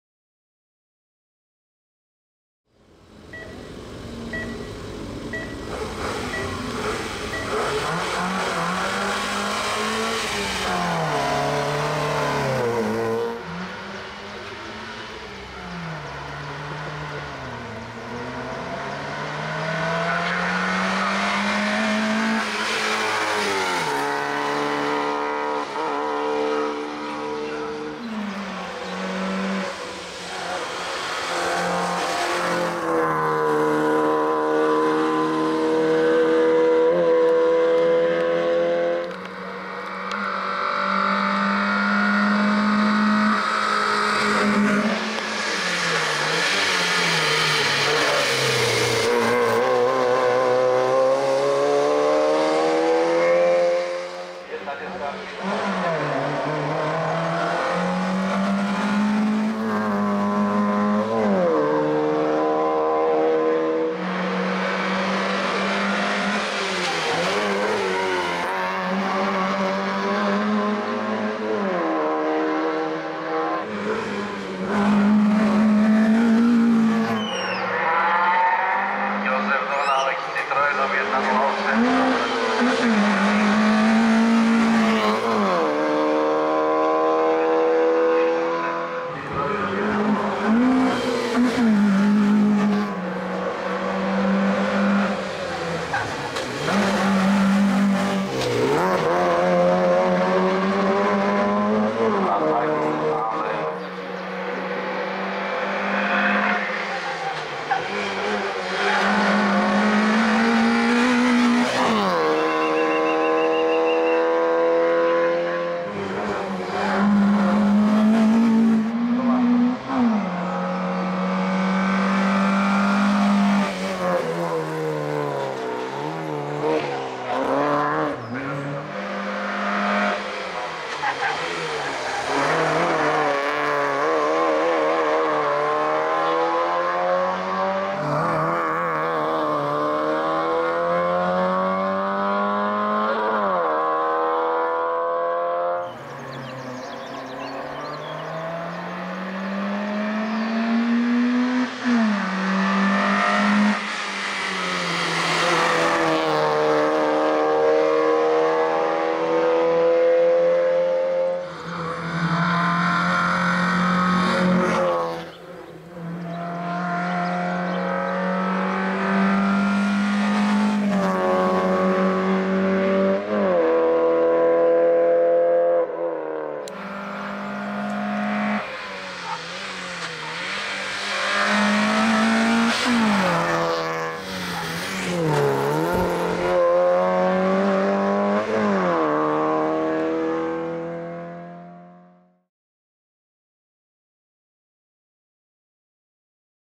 Škoda Fabia - David Oravec - 41. Zámecký vrch 2022 - Náměšť nad Oslavou
David Oravec si s vozem Škoda Fabia dojel pro deváté místo ve třídě E1-1400 v závodech do vrchu 41. Zámecký vrch 2022 v Náměšti nad Oslavou.